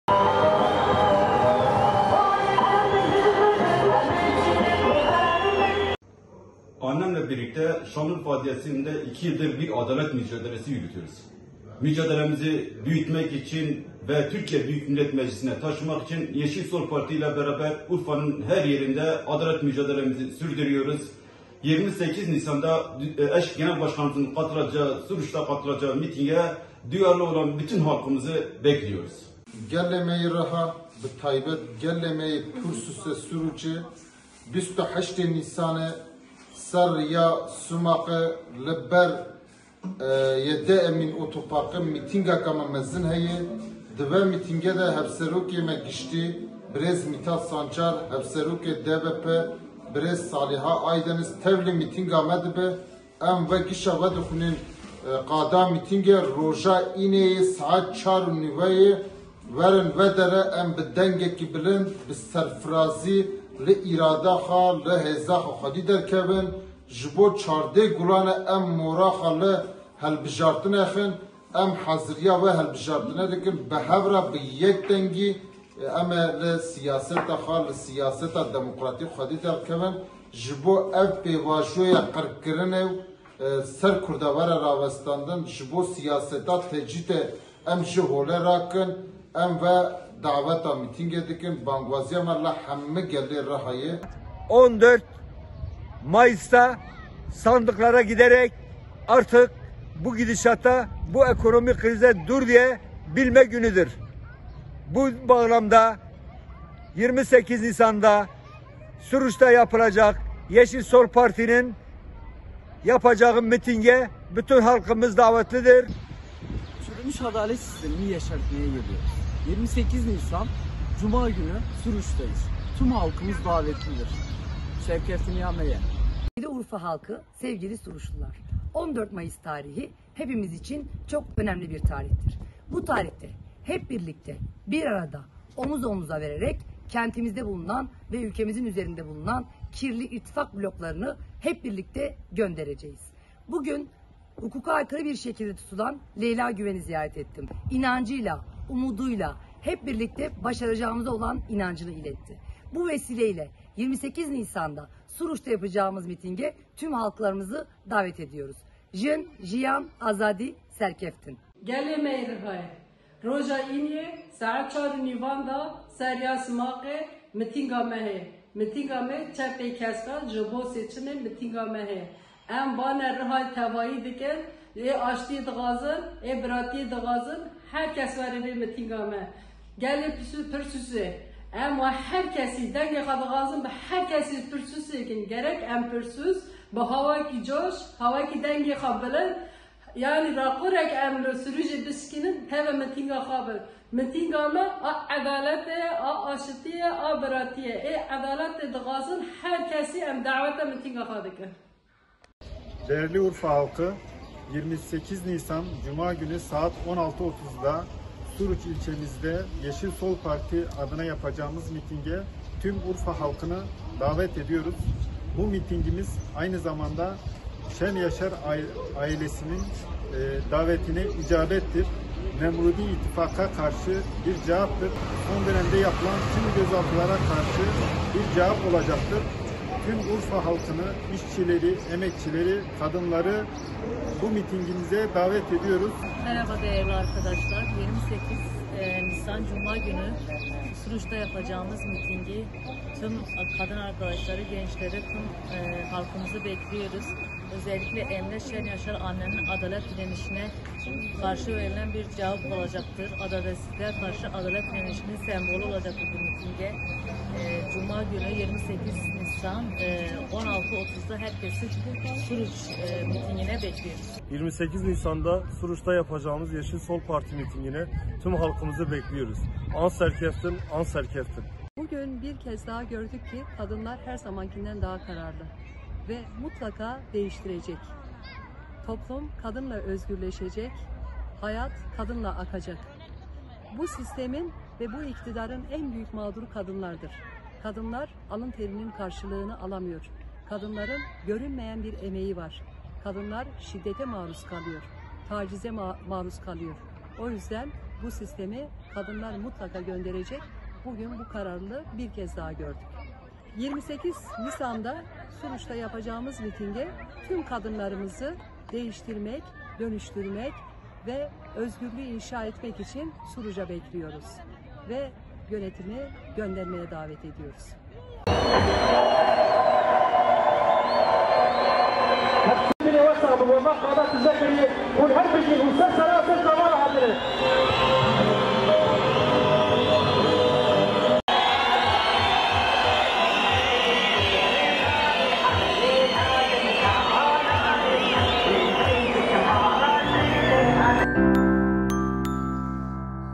PİRSÛS MİTİNGİNE DAVET: YA BAŞARACAĞIZ, YA BAŞARACAĞIZ
Yeşiller ve Sol Gelecek Partisi (Yeşil Sol Parti), Riha’nın Pirsûs (Suruç) ilçesinde 28 Nisan’da gerçekleştirecekleri mitingin çalışmalarını sürdürüyor. Sumak Yolu üzerindeki 7 Emin Otopark’ının yanında gerçekleşecek mitinge, Demokratik Bölgeler Partisi (DBP) Eş Genel Başkanı Saliha Aydeniz, Halkların Demokratik Partisi (HDP) Eş Genel Başkanı ve Yeşil Sol Parti Riha milletvekili adayı Mithat Sancar, HDP Eş Genel Başkan Yardımcısı Tayyip Temel ile Yeşil Sol Parti Riha milletvekili adayları katılacak.

Yeşil Sol Parti Riha milletvekili adayları, 28 Nisan Cuma günü saat 16.30’da gerçekleşecek olan mitinge katılım çağrısı yaptı